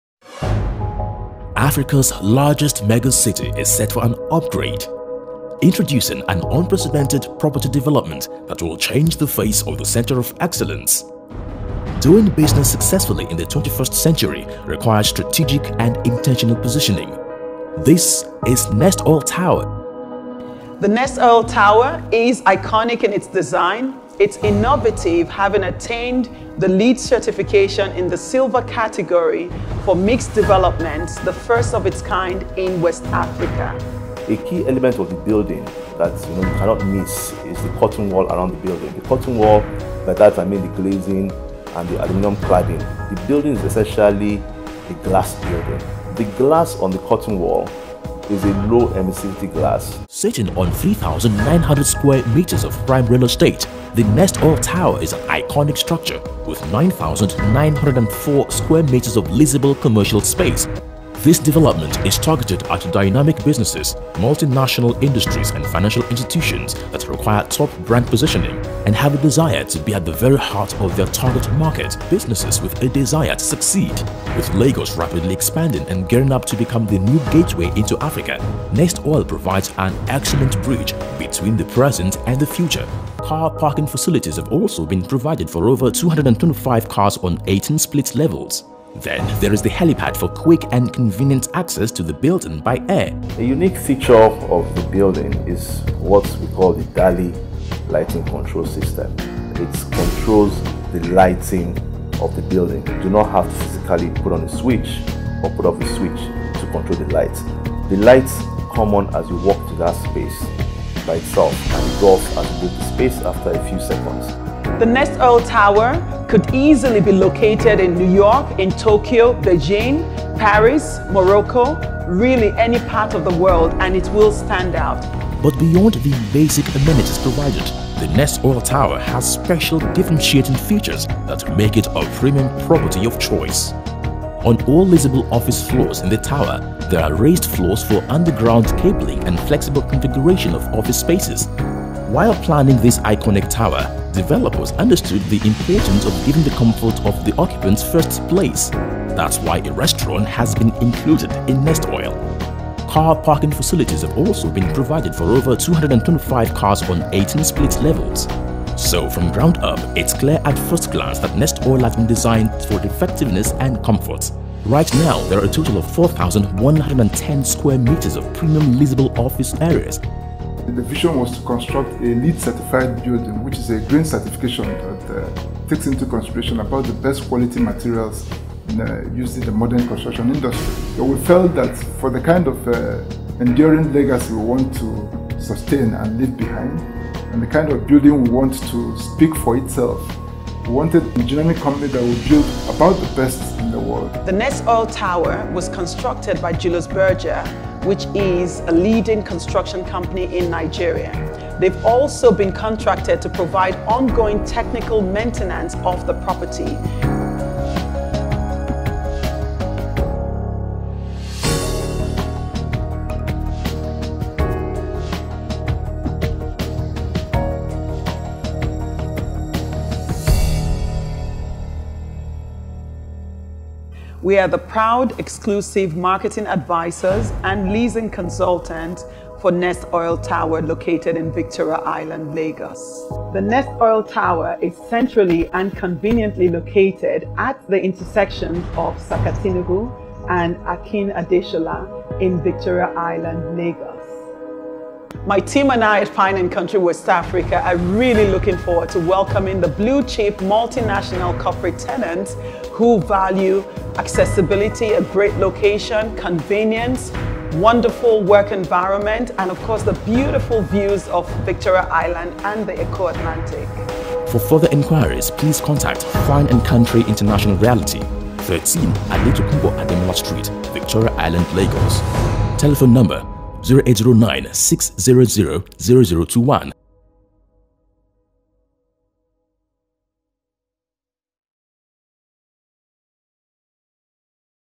NESTOIL TOWER - VICTORIA ISLAND, LAGOS
Nestoil Towers is a world class, LEED Certified, Commercial development which is centrally located in the business district of Victoria Island at the intersection of Akin Adesola and Saka Tinubu streets. Nestoil Tower is Iconic, Innovative and of International Standard.